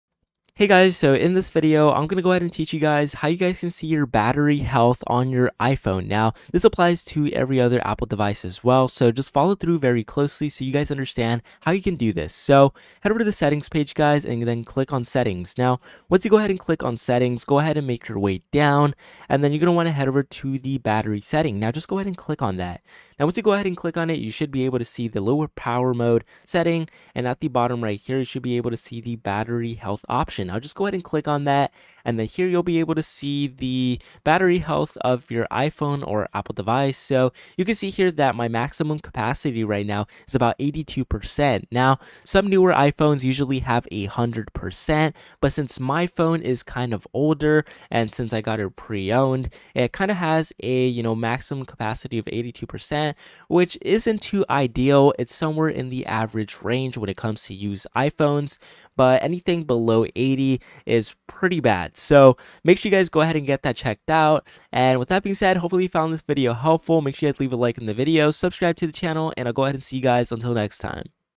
How To Check Battery Health On iPhone
In this tutorial, we'll show you how to check battery health on your iPhone. This is a quick and easy way to get a basic overview of your iPhone's battery life.

If you're needing to know how to get better iPhone battery life, then this tutorial is for you! We'll show you how to check battery health, and then provide some tips on how to get the most out of your iPhone's battery life. By the end of this tutorial, you'll have a basic understanding of how to check battery health on your iPhone, and be able to make the most of your battery life!